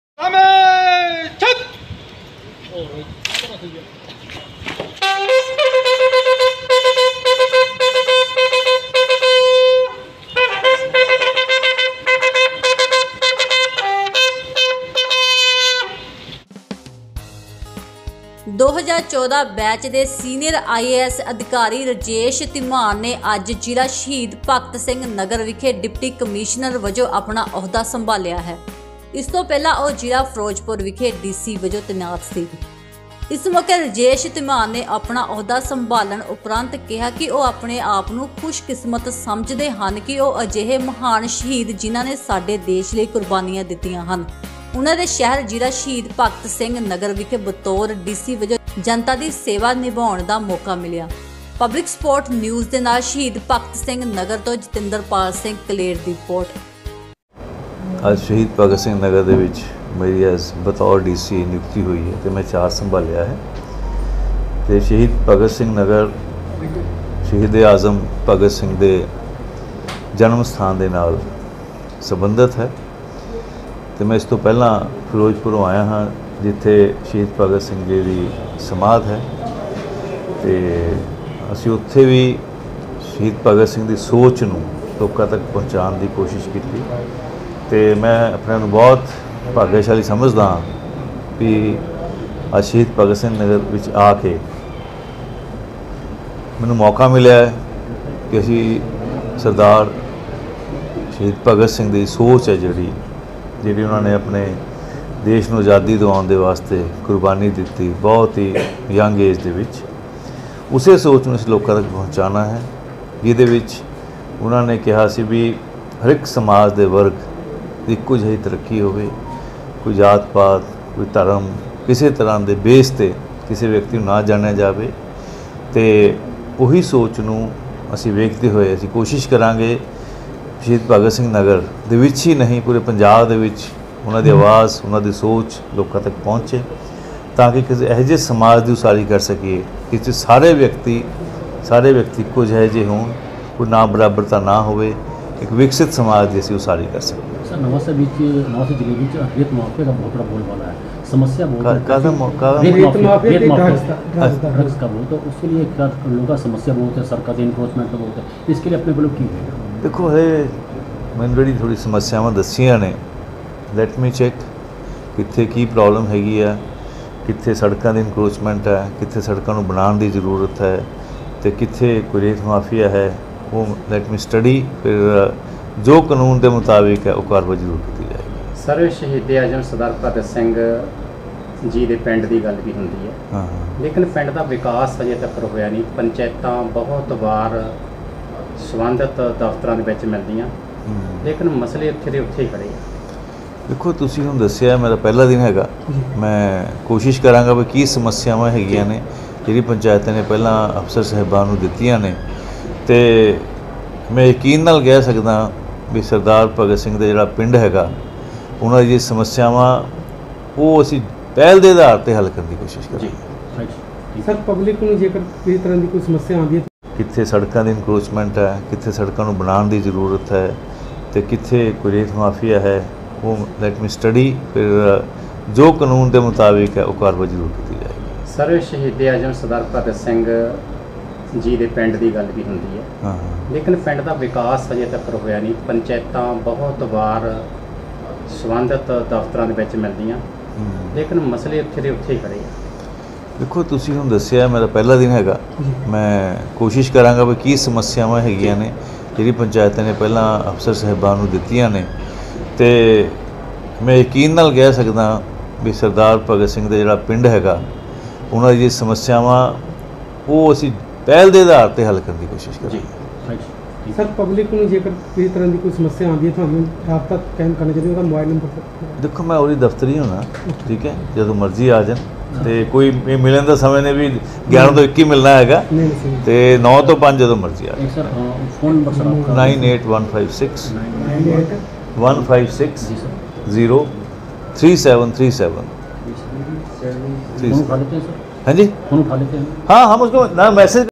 IAS Rajesh Dhiman ਨੇ ਜ਼ਿਲ੍ਹਾ Shaheed Bhagat Singh Nagar ਵਿਖੇ DC ਵੱਜੋਂ ਸੰਭਾਲਿਆ ਅਹੁਦਾ
ਆਈਏਐਸ ਰਾਜੇਸ਼ ਧੀਮਾਨ ਨੇ ਜ਼ਿਲ੍ਹਾ ਸ਼ਹੀਦ ਭਗਤ ਸਿੰਘ ਨਗਰ ਵਿਖੇ ਡੀਸੀ ਵਜੋਂ ਸੰਭਾਲਿਆ ਅਹੁਦਾ